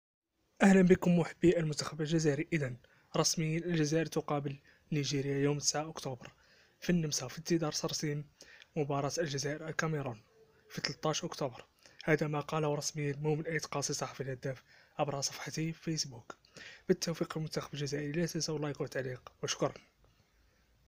رسميا المنتخب الوطني الجزائري يواجه نيجيريا في النمسا. Exclusif . Algérie-Nigeria en amical
رسميا المنتخب الوطني الجزائري يواجه نيجيريا في النمسا